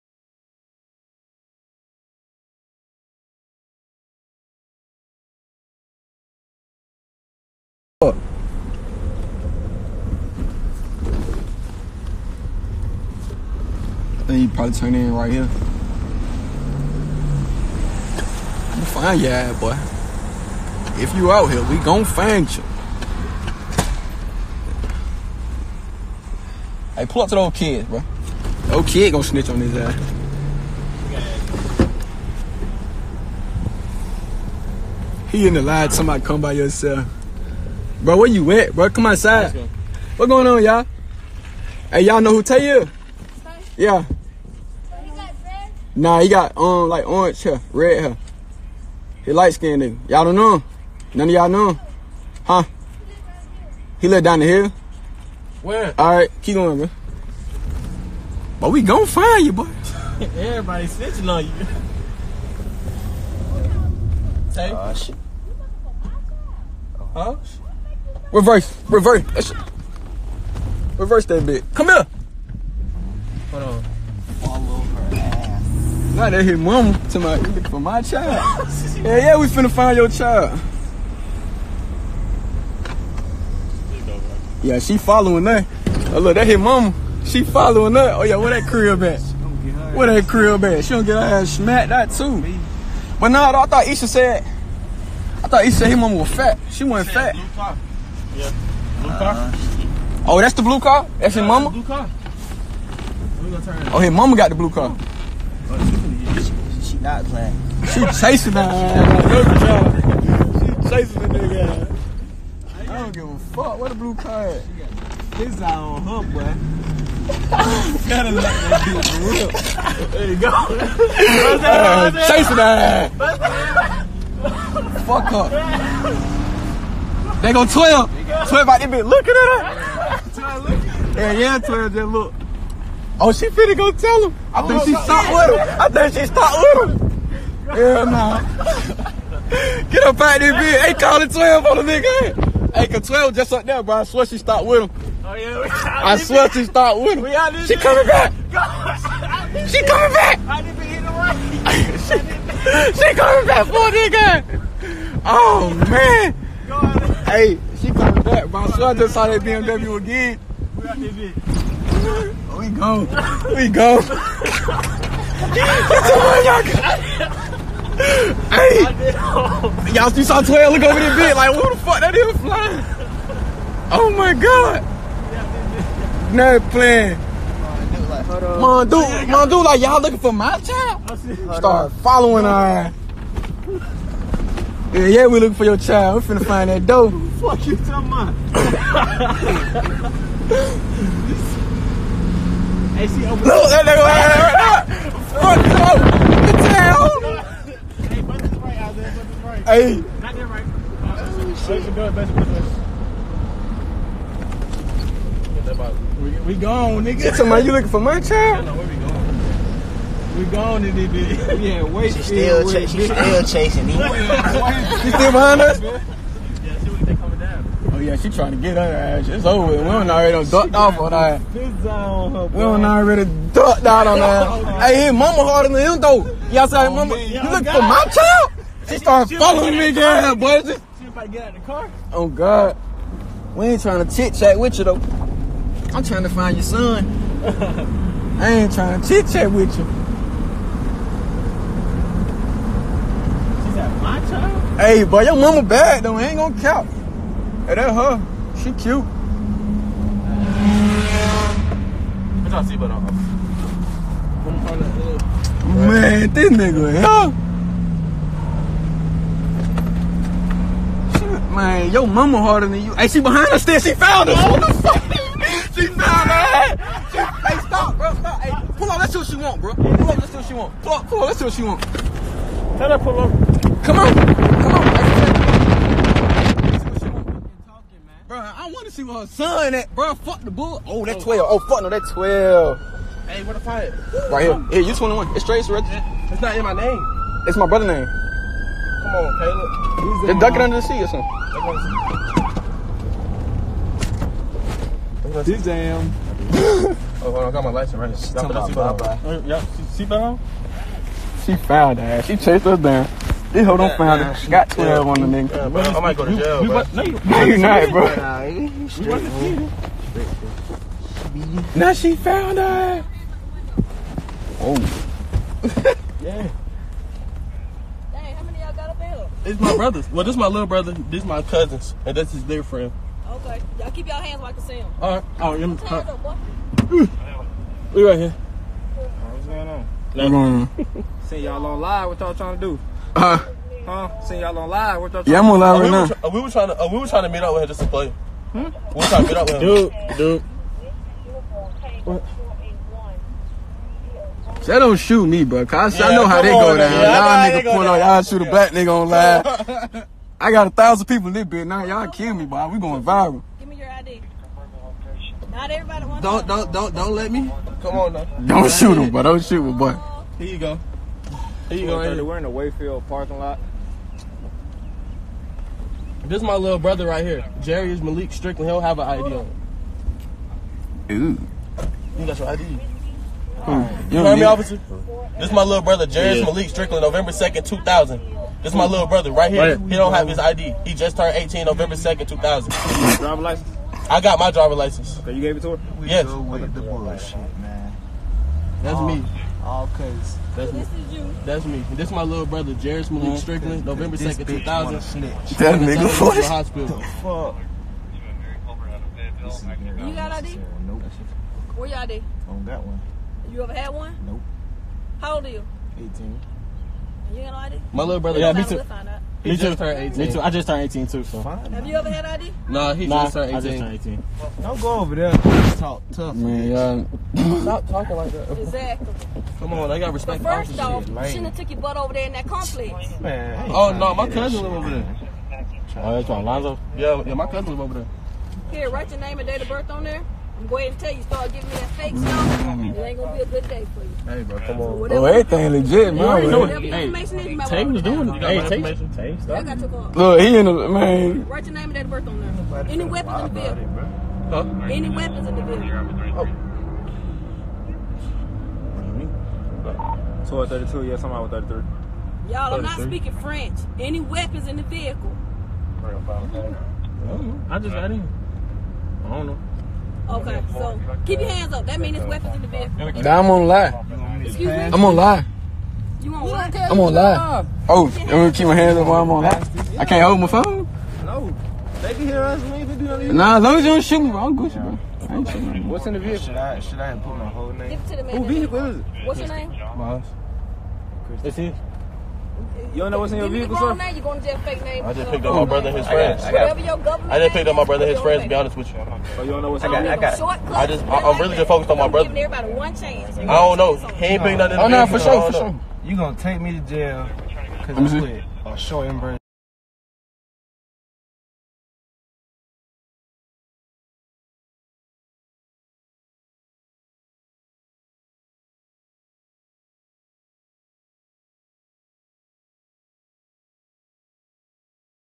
EISHA'S BROTHER GOT PULLED OVER BY THE COPS!!! (pt. 4)
EISHA'S BROTHER GET PULLED OVER BY THE COPS!!! (pt. 4)